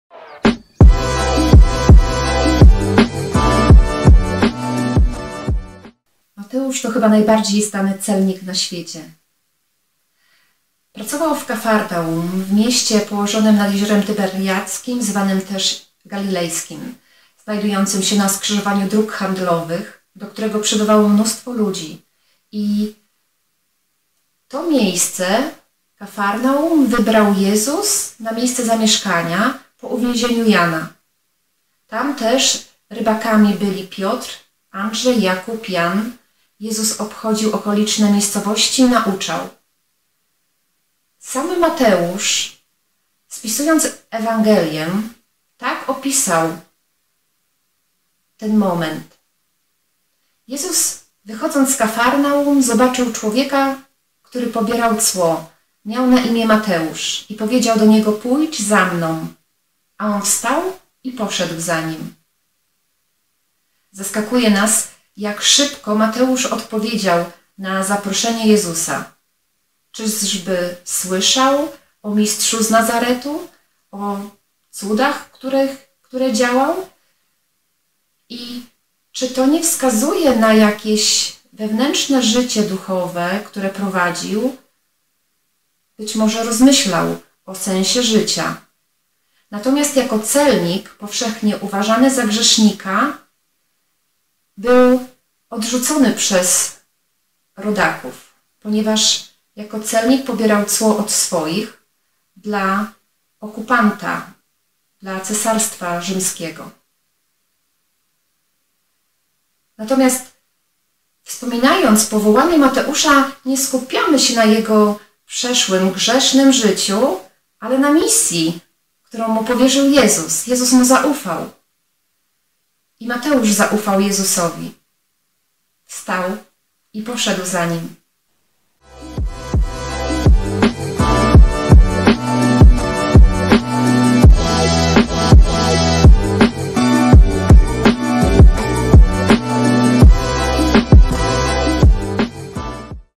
#15 Słowowskaz – Mt 9, 9-13
Zapraszamy na nowy Słowowskaz – o powołaniu Mateusza opowie s. Sylwia Skonieczna FSP

Gdy Jezus odchodził stamtąd, zobaczył człowieka, który pobierał cło. Miał na imię Mateusz. Powiedział do niego: „Pójdź za Mną!”. A on wstał i poszedł za Nim. Gdy Jezus siedział w domu przy stole, wielu celników i grzeszników przyszło i usiadło z Nim oraz z Jego uczniami. Zobaczyli to faryzeusze i pytali Jego uczniów: „Dlaczego wasz Nauczyciel spożywa posiłek wspólnie z celnikami i grzesznikami?”. Gdy On to usłyszał, powiedział: „Lekarz nie jest potrzebny zdrowym, lecz chorym. Idźcie i starajcie się zrozumieć, co znaczy: Miłosierdzia chcę, a nie ofiary. Bo nie przyszedłem powoływać sprawiedliwych, ale grzeszników”.